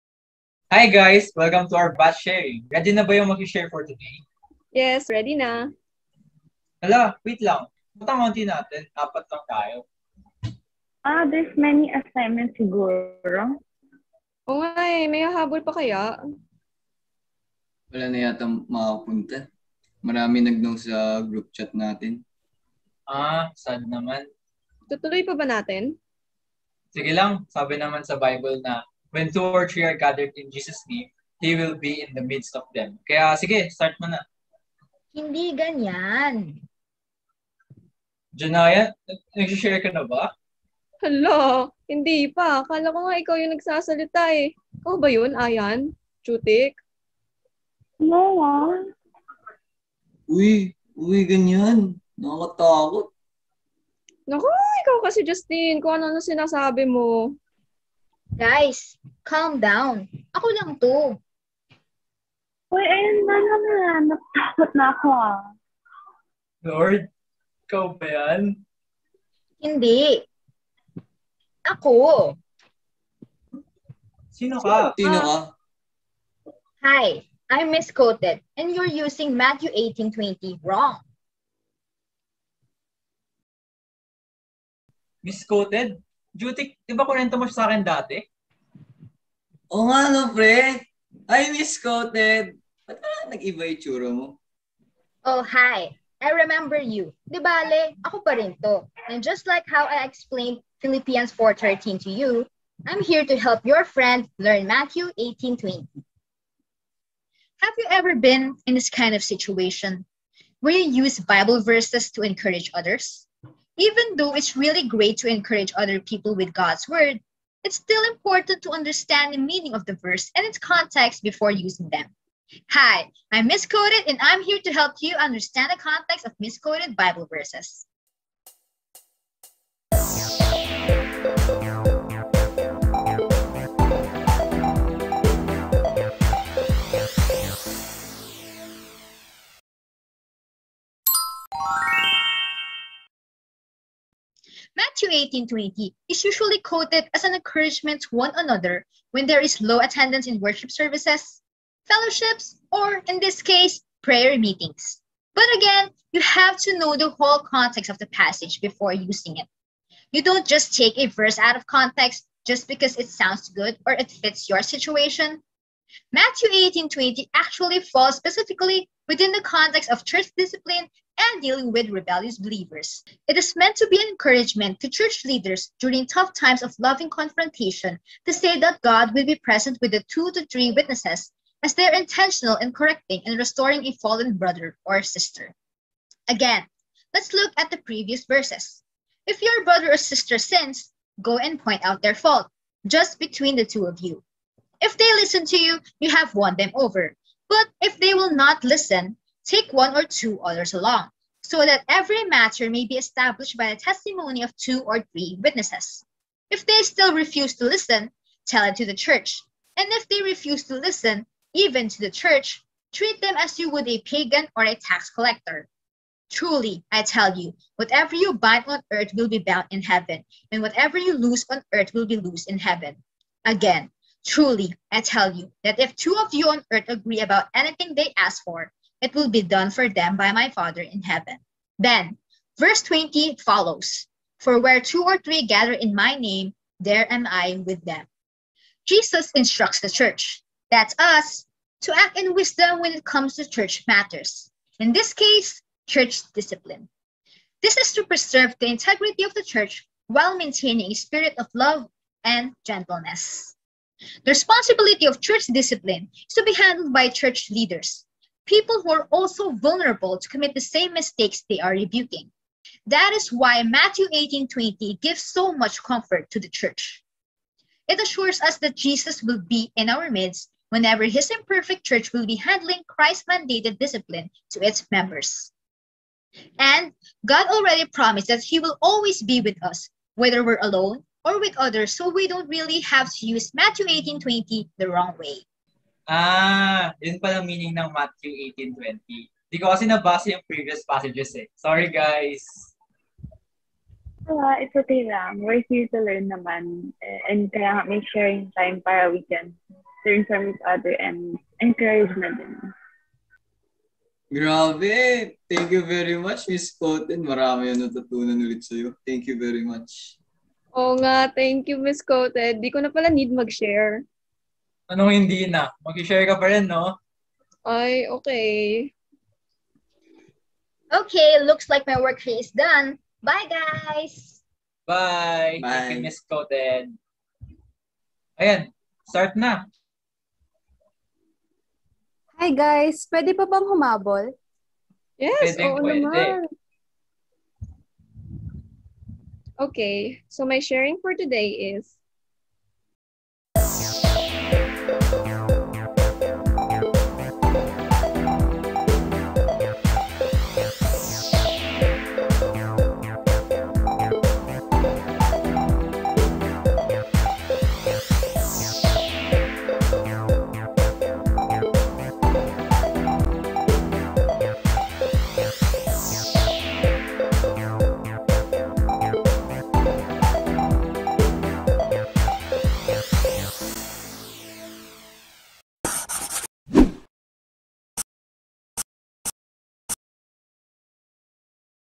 Misquoted Bible Verses | Episode 2 | Matthew 18:20
Matthew 18:20 is usually quoted as an encouragement to one another when there is low attendance in worship services, fellowships, or prayer meetings.

But what does Matthew 18:20 really mean?

In this video, we will discuss the real meaning of the verse and its context so that we can finally stop misquoting this particular Bible verse. 😉

Special thanks to these amazing people for making this video possible 🥰